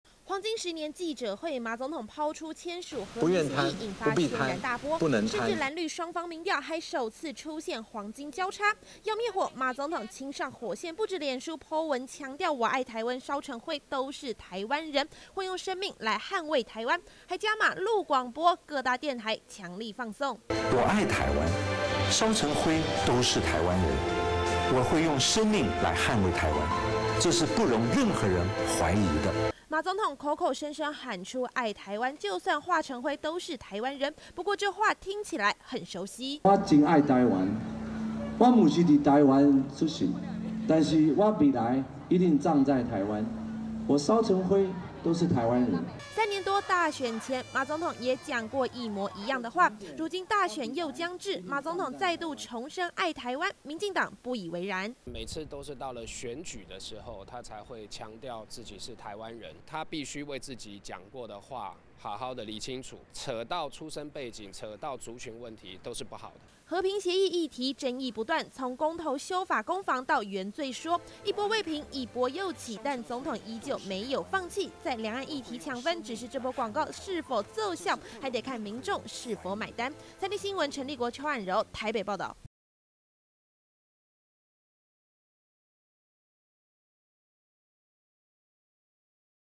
馬:化灰也是台灣人 綠譏:選舉語言
馬總統日前在黃金十年記者會上，拋出簽署"兩岸和平協議"議題，不但沒能在選戰中得分，還引發是否支持公投的藍綠論戰，不過要滅火，馬總統又有新招，不只在臉書PO文，強調自己就算化成火，還是台灣人，最近，更錄製國台語雙聲版廣播，在電台強力放送，不過，民進黨諷刺，馬總統是對自己沒信心，才一再強調自己是台灣人！